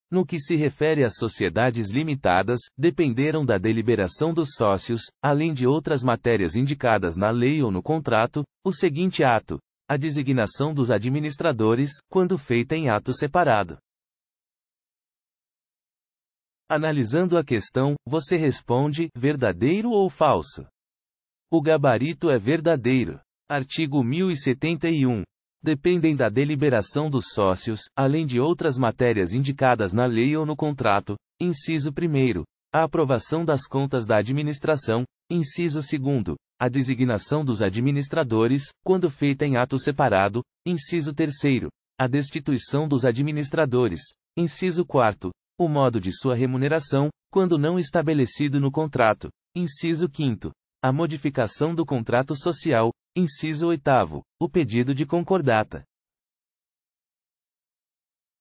QScon Direito: [Art. 1.071, CC] IESES 2014 - Notário / Registrador (TJ-PB)
Art. 1.071, CC | IESES 2014 | Notário / Registrador | (TJPB)

No que se refere às sociedades limitadas, dependerão da deliberação dos sócios, além de outras matérias indicadas na lei ou no contrato, o seguinte ato: A designação dos administradores, quando feita em ato separado.

Gabarito: Verdadeiro